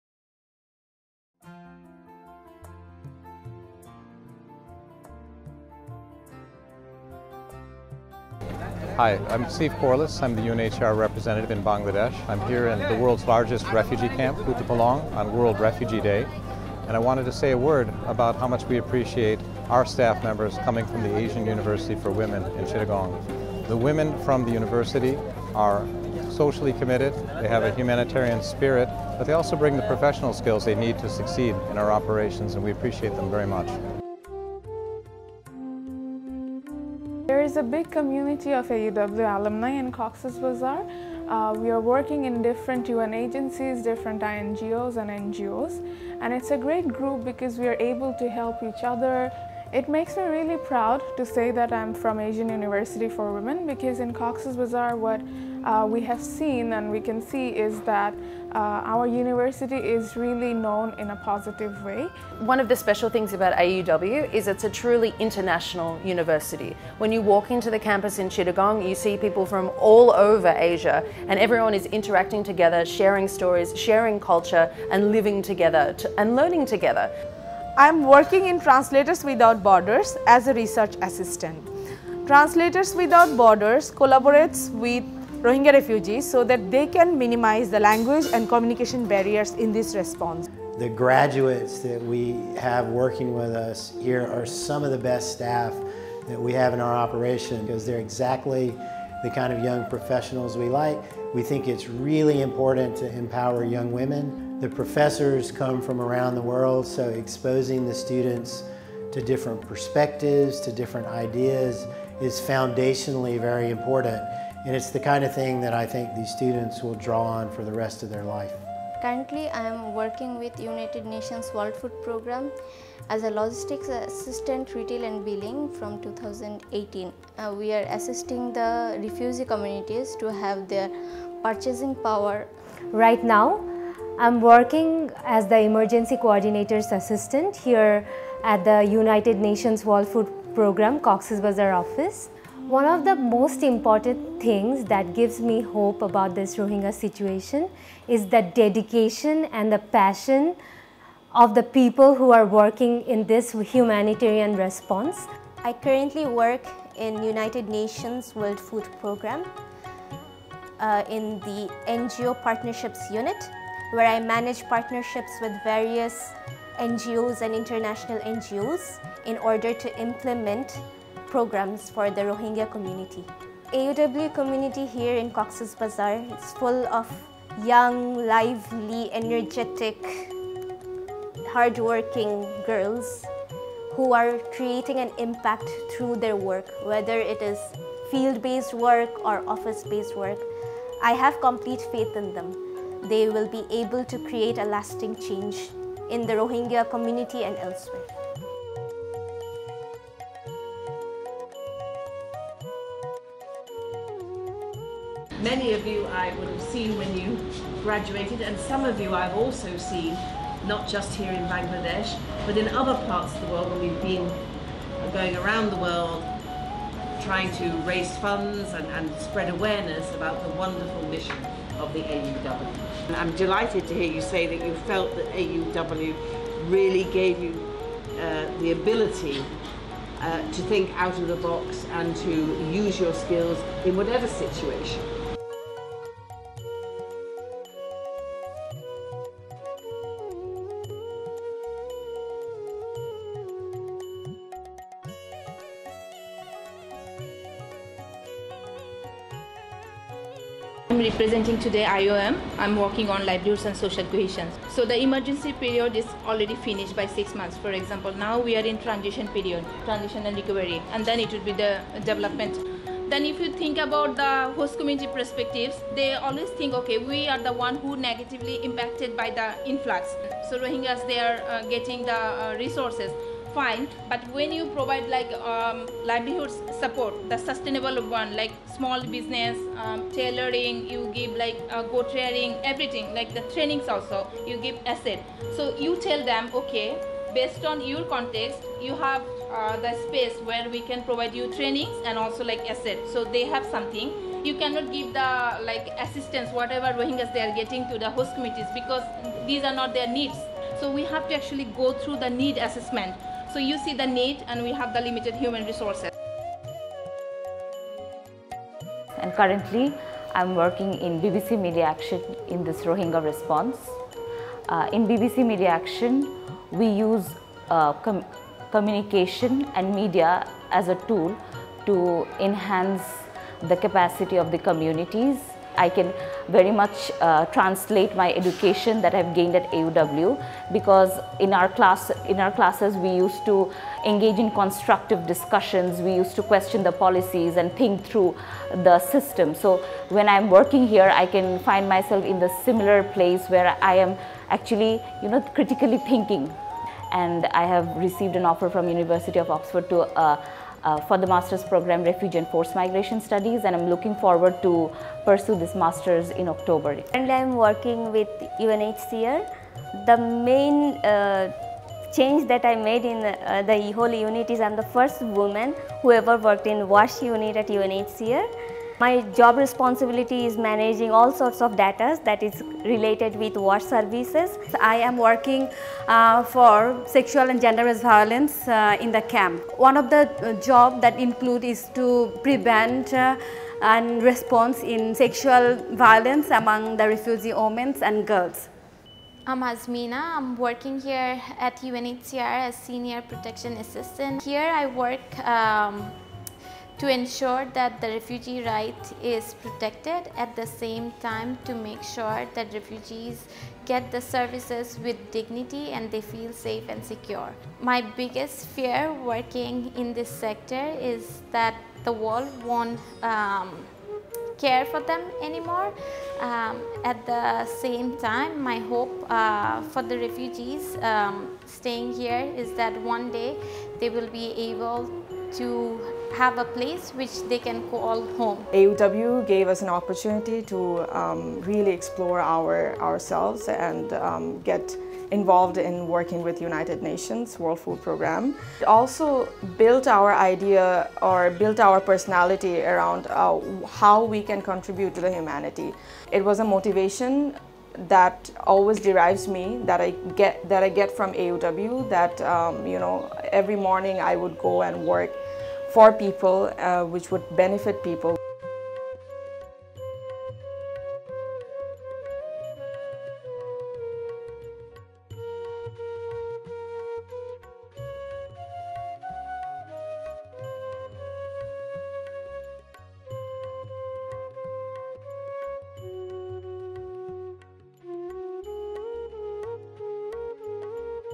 Rohingya Refugee Crisis: AUW Alumnae at the Forefront of the Response
Over 1 million Rohingya refugees reside in camps in Cox's Bazar, Bangladesh.

On Thursday, June 20, the world commemorated World Refugee Day. In honor of this occasion, Asian University for Women is releasing a new video about the more than 45 AUW graduates supporting the Rohingya refugees sheltered in Cox's Bazar, Bangladesh as employees of the UN and other associated organizations.